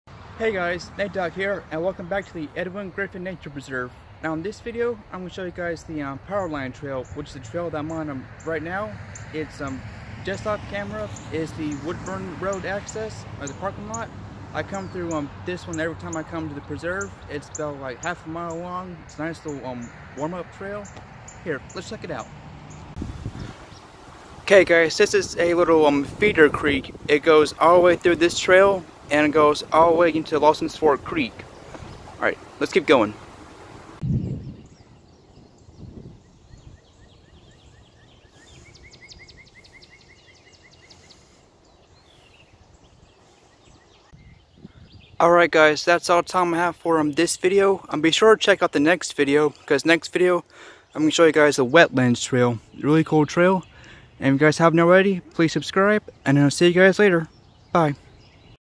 NATEDOG shows you the Edwin Griffin nature preserve (part 5)
Check out this cool trail!